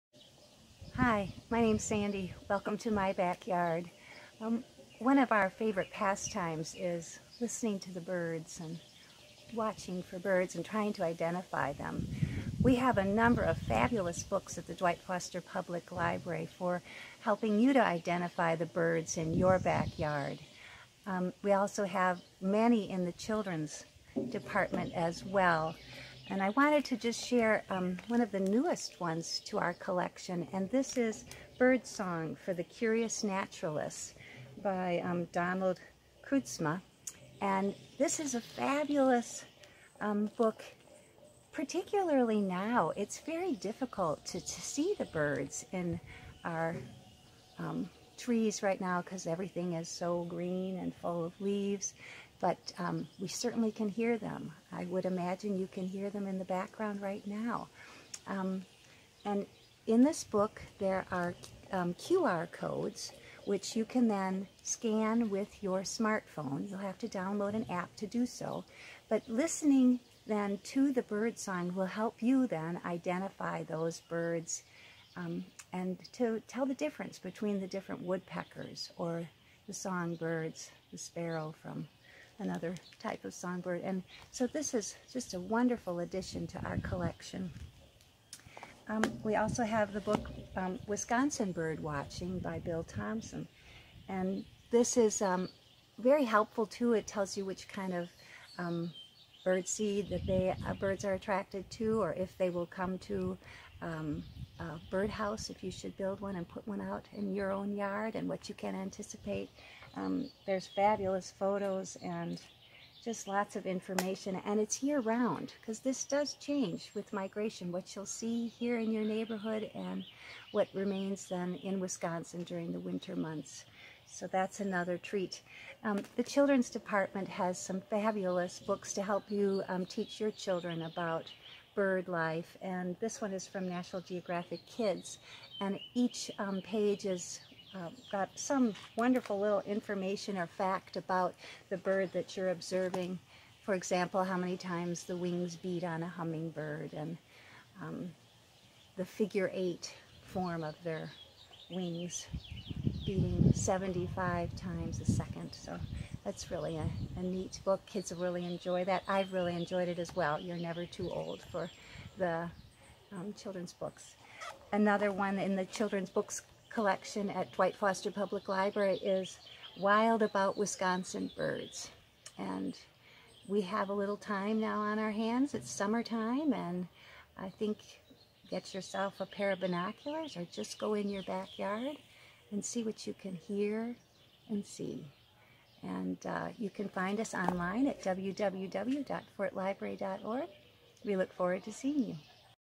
Books for Bird Lovers
Sandy reviews several books of interest to bird watchers.